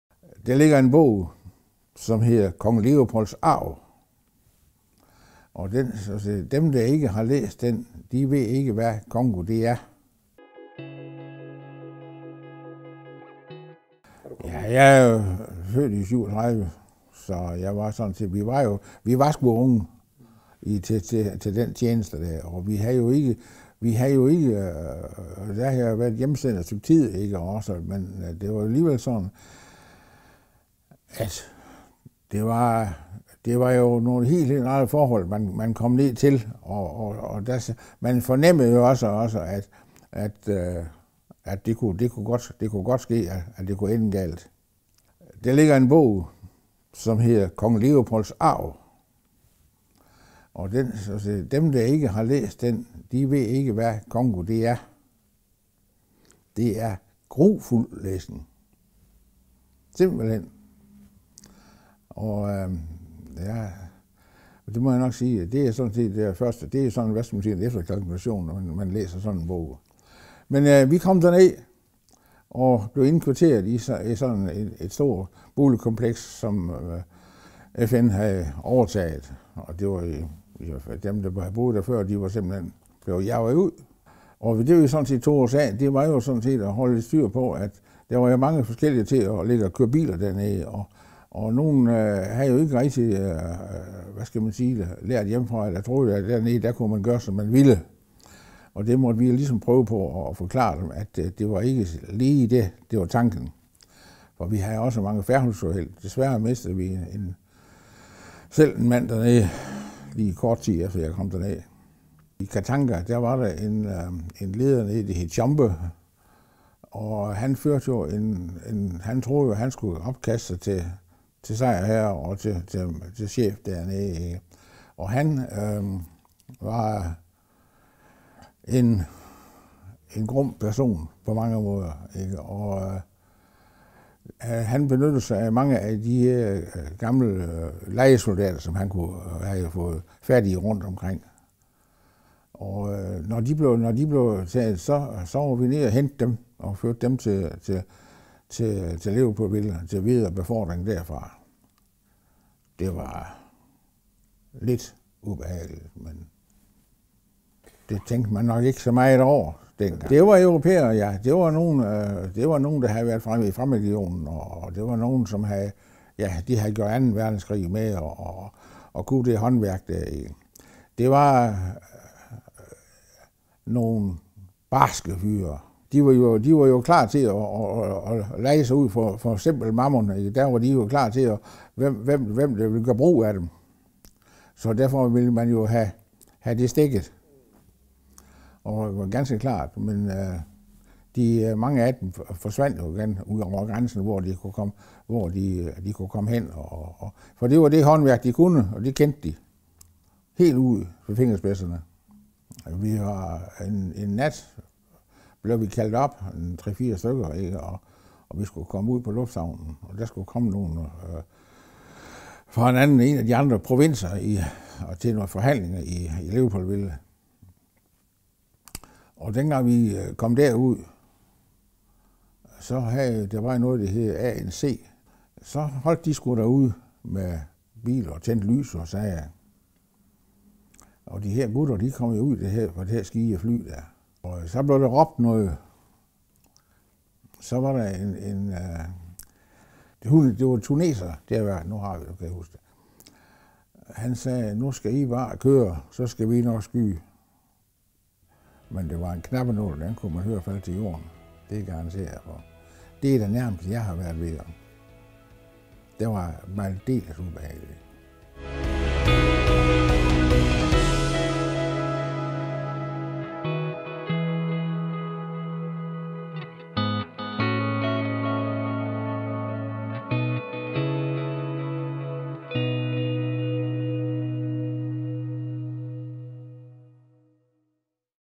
MP i Afrika
Kjeld ryger afsted til Congo i 1962. Han havde været hjemsendt nogle måneder og ryger afsted uden anden form for træning end den, han havde fået fra MP-skolen.

Tekst af Ann-Christina Salquist

Kjeld ryger afsted til Congo. Der er opstået borgerkrig efter at Congo i 1960 efter flere års belgisk styre opnår selvstændighed.

I tiden under Kong Leopold var Congos indbyggere blevet holdt i et yderst stramt og brutalt greb, og da landet bliver selvstændigt, står congoleserne med et land, der er udnyttet – både menneskeligt og ressourcemæssigt.

Den Demokratiske Republik Congo (herefter omtalt DR Congo) var indtil 1908 den belgiske kong Leopold II’s personlige ejendom. I 1908 tog den belgiske regering over og regerede landet, indtil det fik sin selvstændighed i 1960. Kong Leopold og den belgiske regering var ansvarlig for et af verdens mest brutale koloniregimer, og da belgierne trak sig ud, efterlod de sig groft udnyttet land med store interne konflikter og dårlig infrastruktur.

Sammen med en række af fra militærpolitiet rejser Kjeld ned til Congo for at holde styr på den store FN- styrke, og de kæmper en kamp med at holde ro og orden – både på egne styrker, men også de lejesoldater, som Tschombe hyrer og får til at udføre grusomheder.

Vil du høre en anden historie fra Congo, anbefaler vi, at du hører Thøger fortælle i ‘Kannibaler‘.

Del fortællingen med dit netværk